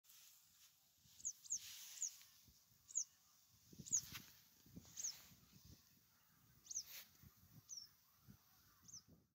Oostelijke gele kwik
Roep van de (mede) door ons ontdekte Oostelijke gele kwikstaart van Paezemerlannen.